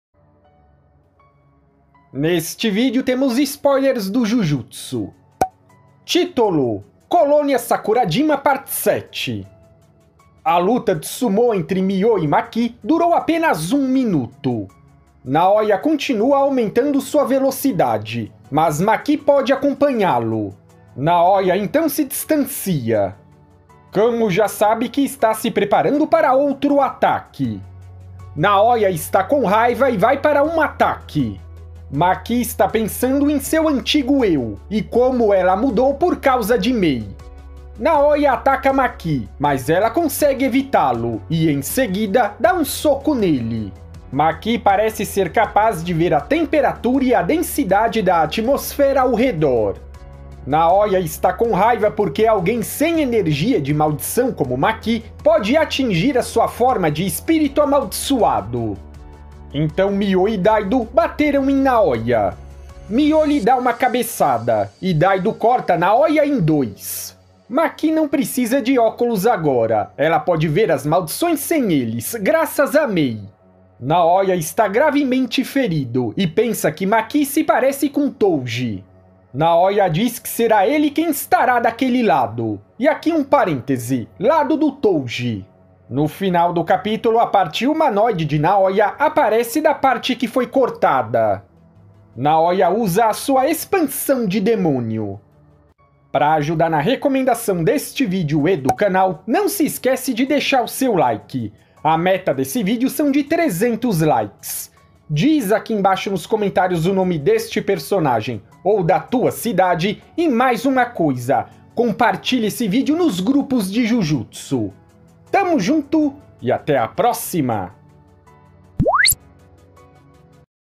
BOMBA! Jujutsu Kaisen 197 - NAOYA FORMA HUMANA ESTÁ DE VOLTA!!!
jujutsu kaisen 197 spoilers
Spoiler Chapter 197 Jujutsu Kaisen
jujutsu kaisen 197 resumo

spoilers foram vazados, neste capítulo veremos MAKI vs NAOYA